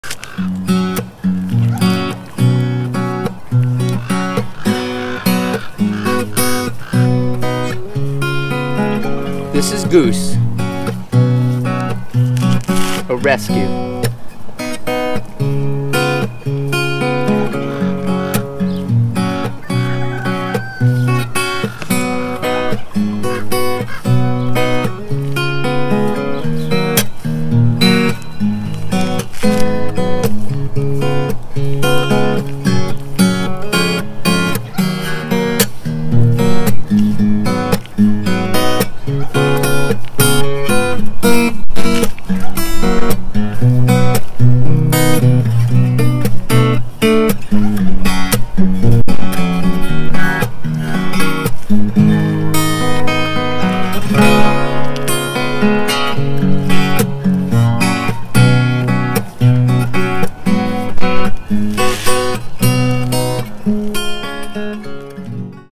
His name is Goose and he loves music.
Why not be cleaned while playing for Goose.